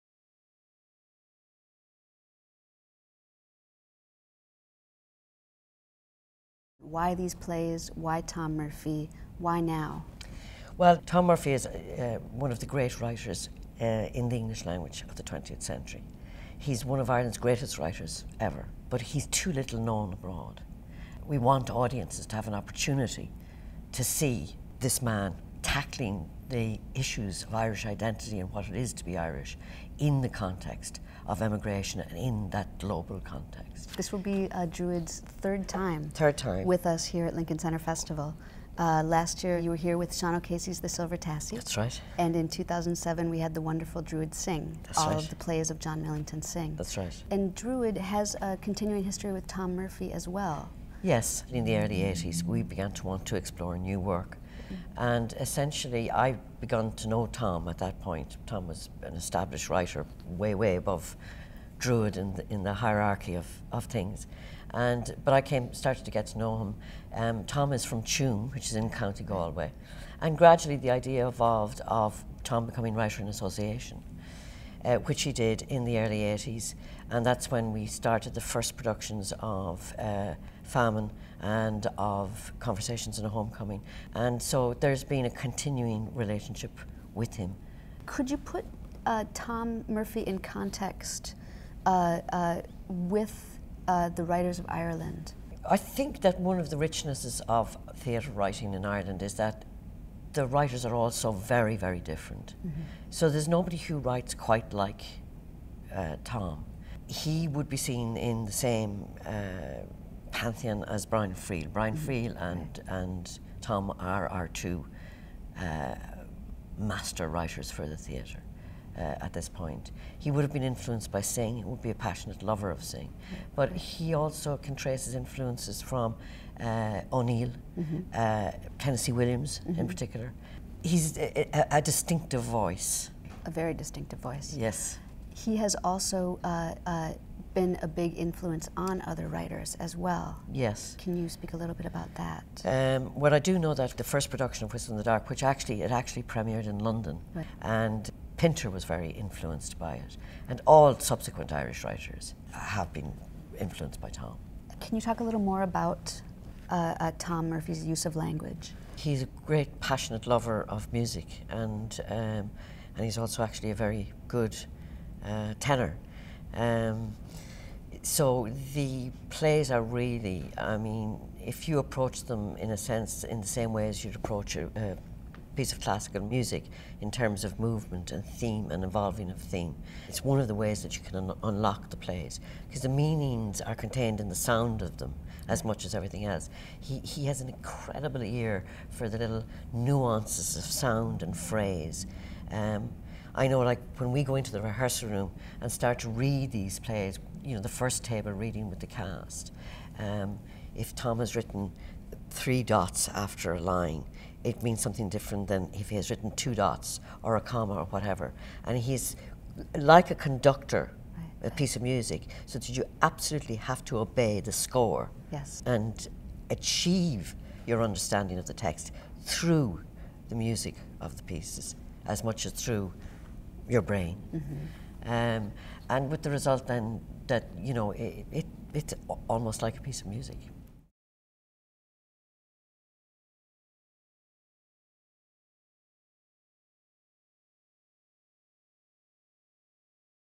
Lincoln Center Festival 2012: Tom Murphy The Playwright
Crossing an ocean and spanning over a century, DruidMurphy is the story of Irish emigration, of those who went and those who were left behind—a sweeping tale told through three great plays by Tom Murphy: Conversations on a Homecoming, A Whistle in the Dark, and Famine.
DruidMurphy joins Lincoln Center Festival from July 5 through July 14.